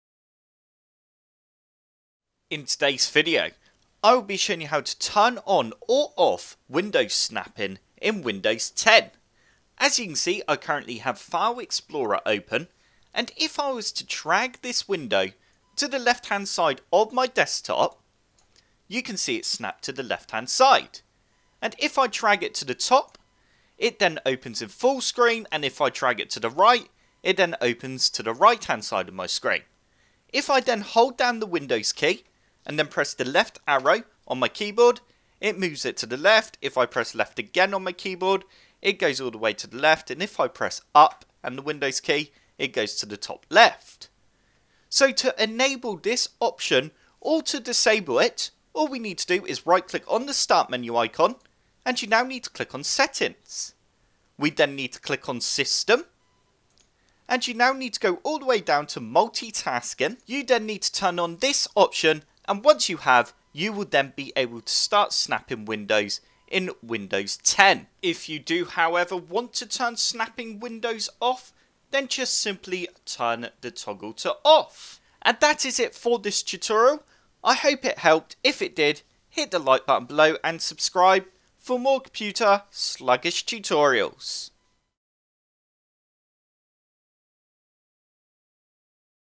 How To Turn Snapping Windows On or Off in Windows 10 Tutorial
In this Windows 10 tutorial, I will be showing you how to turn snapping on or off if you are unsure what snapping is it basically allows you to drag windows to the edge of your screen, and it will then resize and snap the window into place allowing you to then snap another window next to it I will show you at the start of the video an example of snapping.